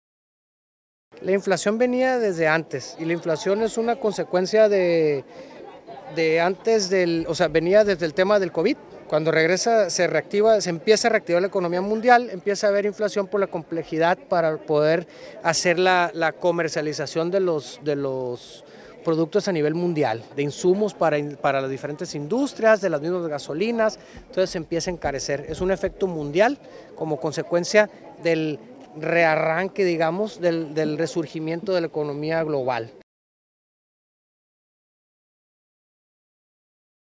Efectos de la inflación, se tienen que aprovechar para traer más industria a México: Gaxiola Coppel
La inflación es una consecuencia de pre y pos pandemia y sus efectos, se sienten no solo en la economía de México, sino a nivel global, consideró el Secretario de Economía en Sinaloa, Javier Gaxiola Coppel.

Mientras tanto, estos comportamientos en la economía se tienen que aprovechar para traer más industria al país y no se presente el problema de logística que está ocurriendo actualmente, sobre todo, por la vía marítima.

Pero también, durante su estancia en el puerto de Mazatlán, el funcionario estatal habló de otro problema que se está presentando, particularmente en la industria automotriz y es el déficit de unidades que se registran, debido a la escasez de microchips.